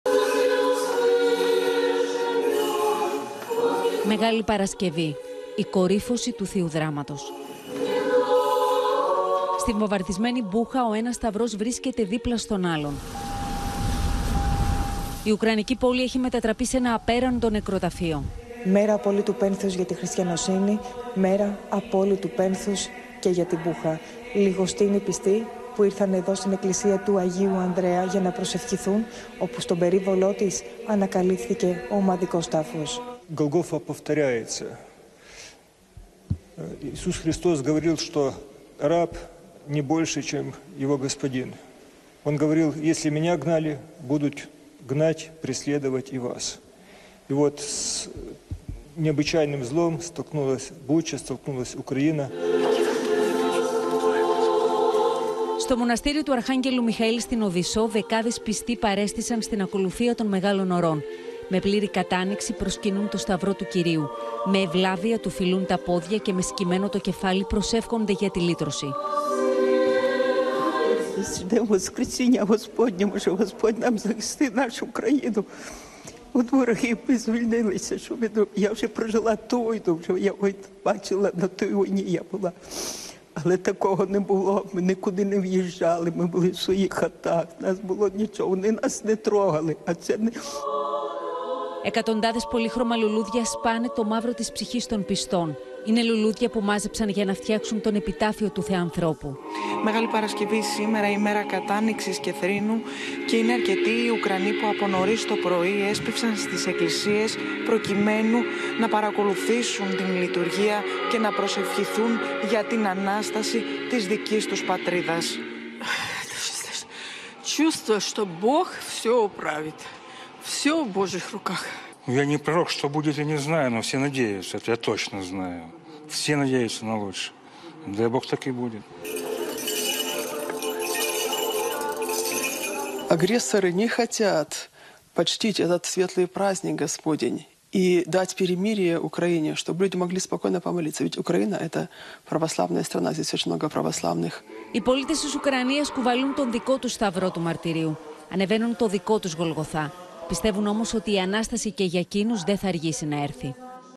Τον δικό τους «σταυρό του μαρτυρίου» κουβαλούν οι κάτοικοι της Μπούχα | Κεντρικό Δελτίο Ειδήσεων
Μία Μεγάλη Παρασκευή διαφορετική από τις άλλες είναι για τους Ουκρανούς πιστούς. Παρά τον πόλεμο, όμως, πολλοί παρακολούθησαν τη Λειτουργία στους ορθόδοξους Ναούς στην Μπούχα και στην Οδησσό και προσευχήθηκαν για τη δική τους Ανάσταση.